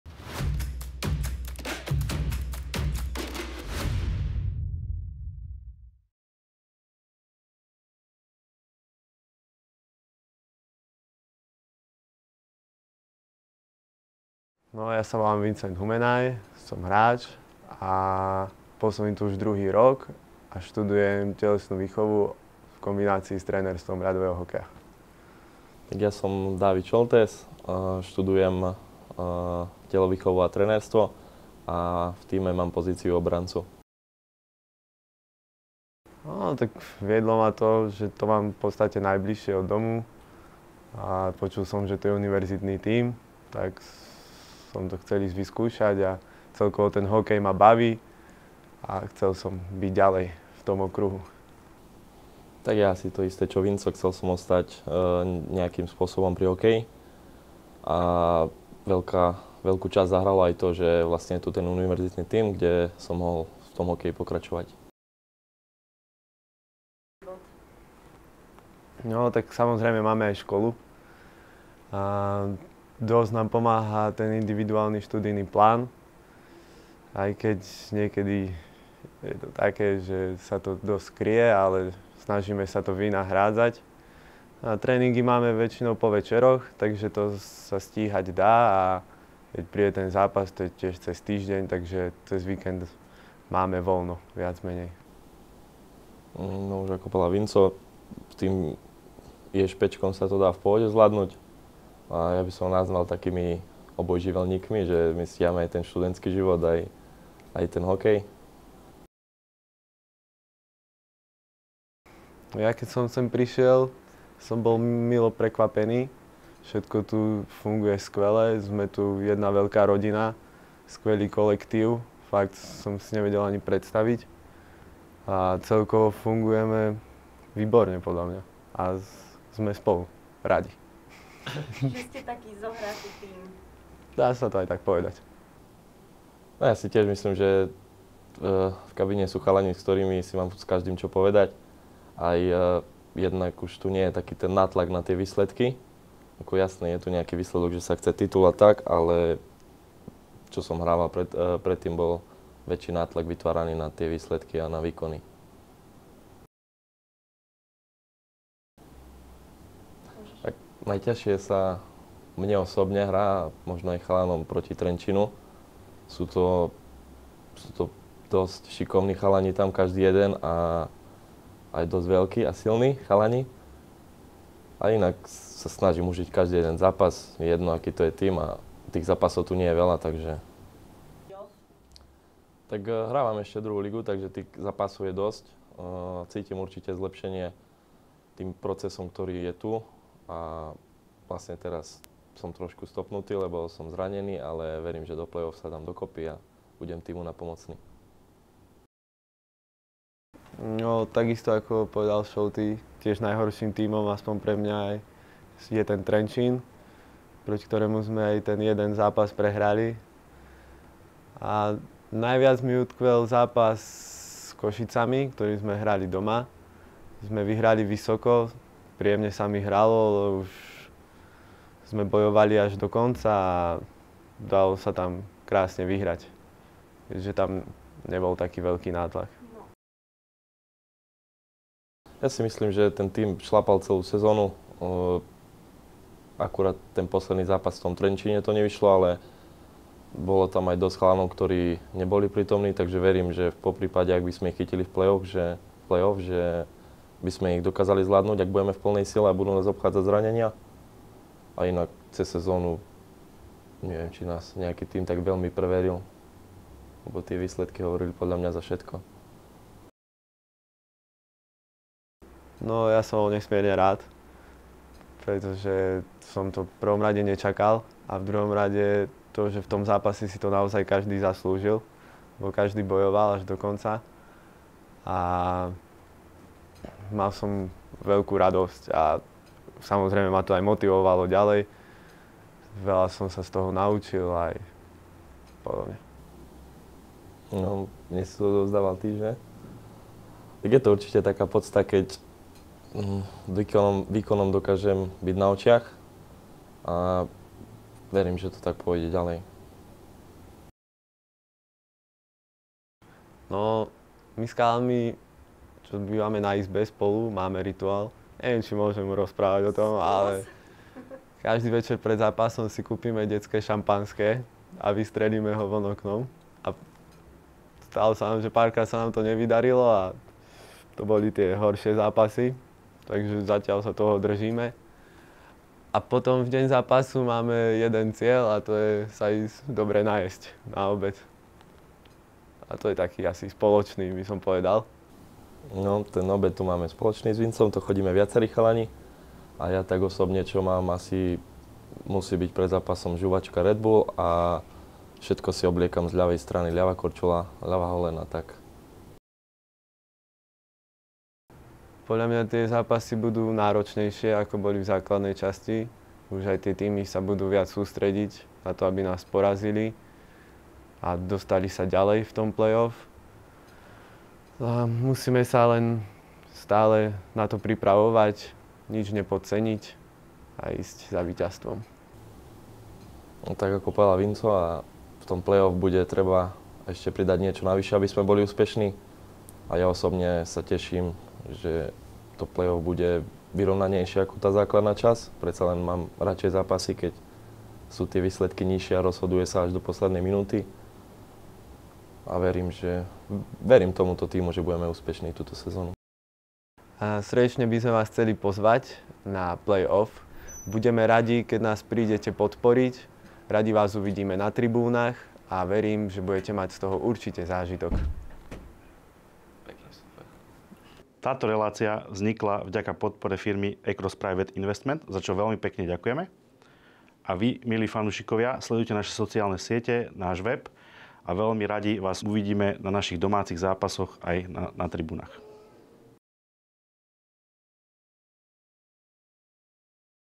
Zo šatne #7 2022-2023 (Šoltés & Humenaj) - Partner: Across
Diel 7
Zo šatne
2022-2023

Relácia zo šatne, z hokejovej kuchyne UMB Hockey team Banská Bystrica. Detaily diania, príbehy hráčov, informácie o lige.

PARTNER RELÁCIE: Across Private Investments